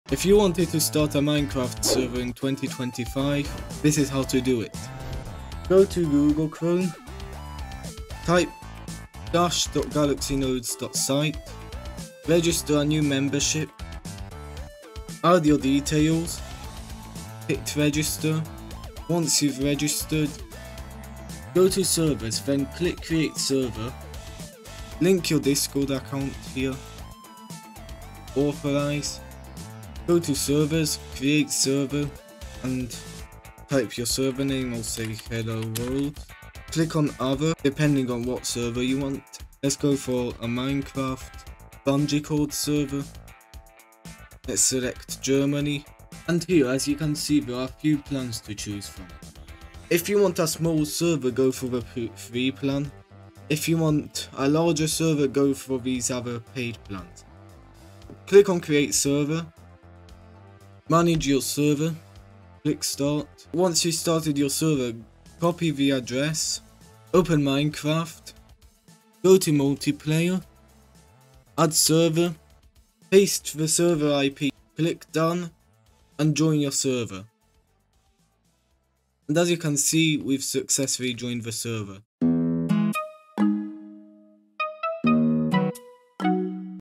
NEED ATERNOS? Use this FREE Minecraft Server Hosting in 2025! | SodiumNodes
Learn how to make a free Minecraft server in 2025 with GalaxyNodes! Follow our easy step-by-step tutorial to start playing with your friends today.